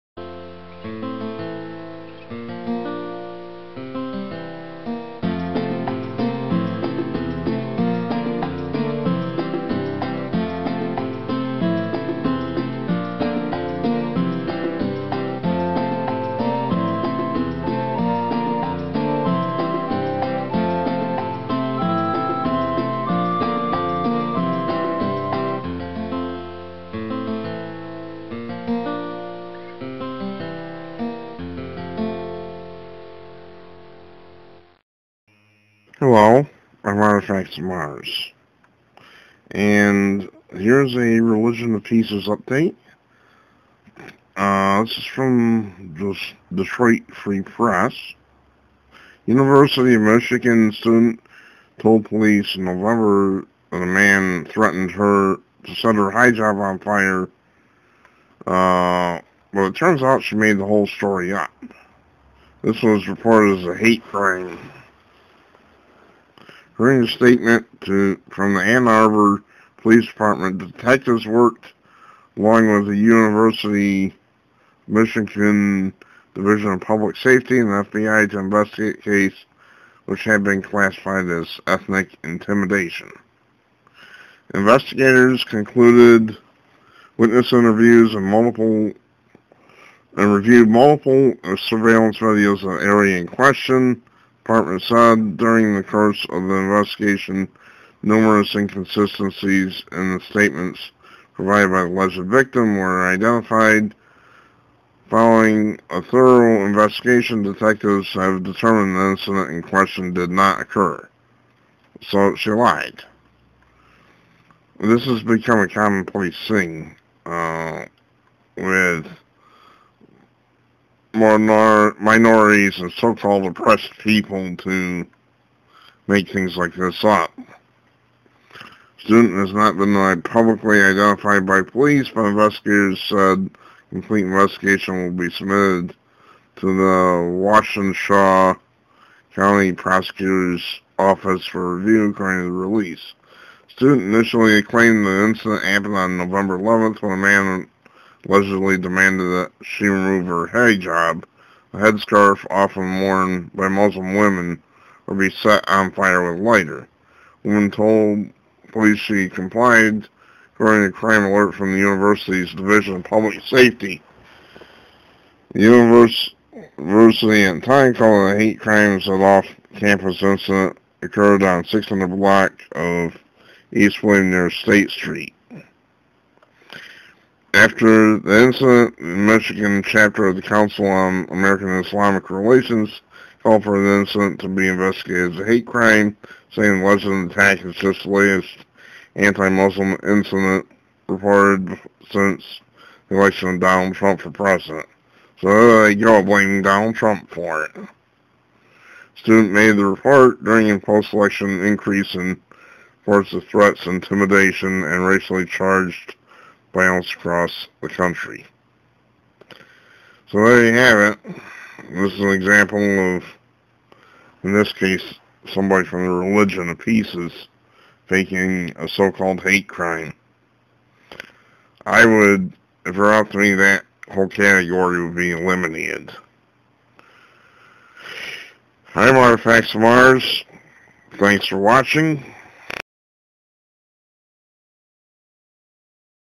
Muslim woman said that man told her to remove Hijab or be burned Liar!
A Muslim student reported to campus authorities that a man ordered her to remove her Hijab or else he would set her on fire with a lighter. Accepted at first as a hate crime, this has been dismissed as a fake hate crime by Michigan police.